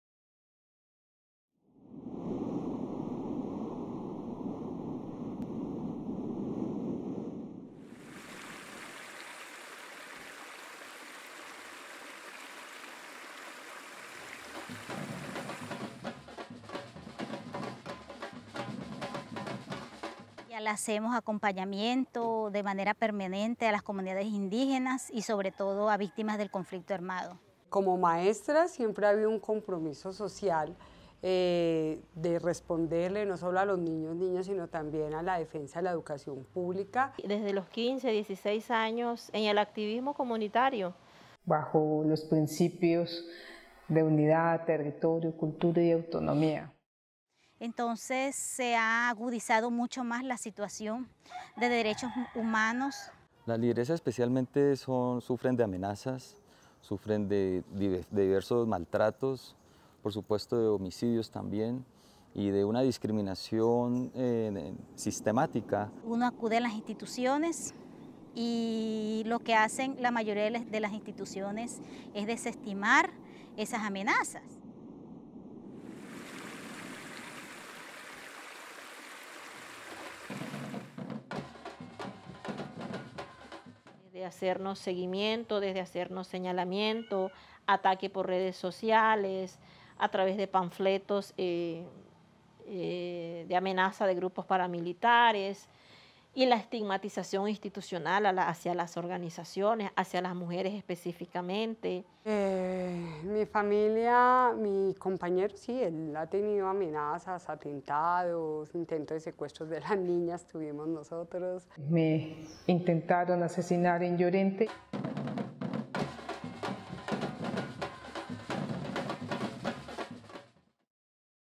Colombia: mujeres bajo ataque
Cada 29 horas una mujer es agredida en Colombia por defender los derechos humanos, mientras los responsables por estos hechos gozan de casi total impunidad.